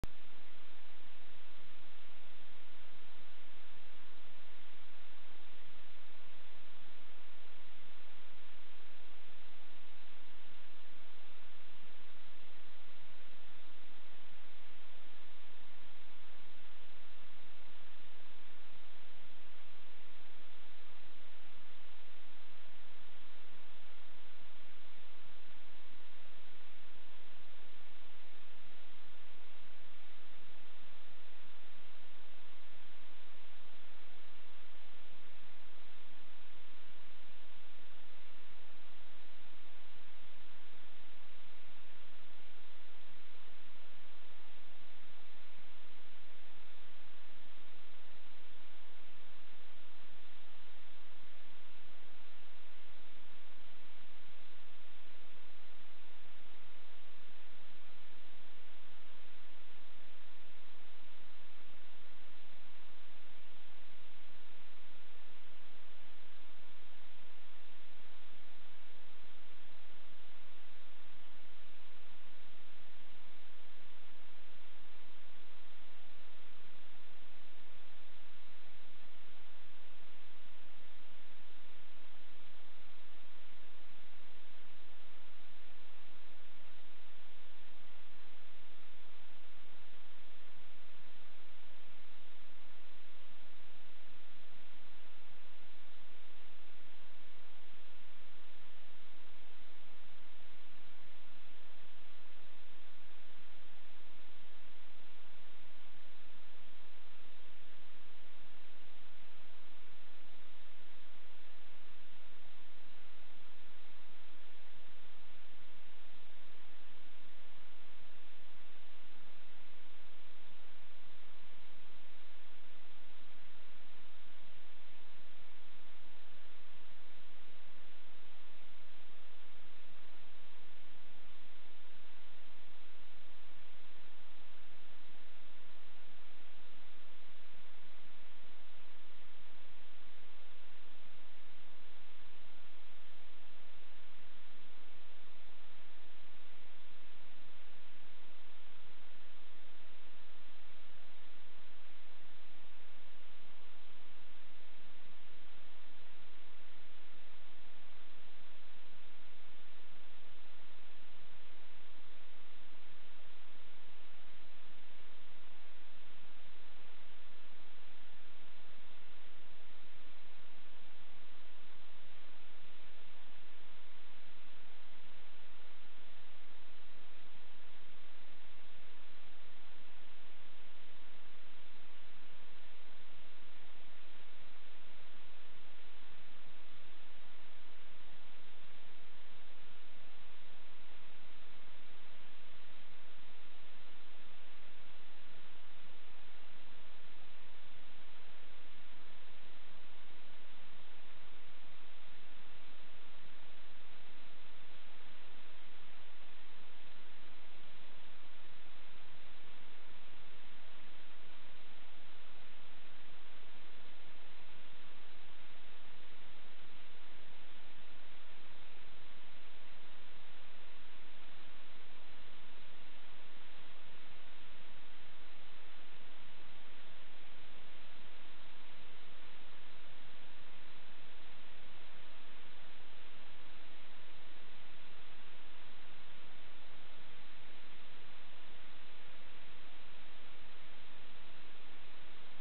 avec pid modifier
un regale pour un noubSSSSSSSSSSSSSSSmaudif pid.lx5 esck 4/1 45f dys,motuer ld power 2307/2600kv,frsky f40 ,taranis x7 ,foxeer predator v2 tout ca enregistre via dvr sur google two!je progresse la enfin!!!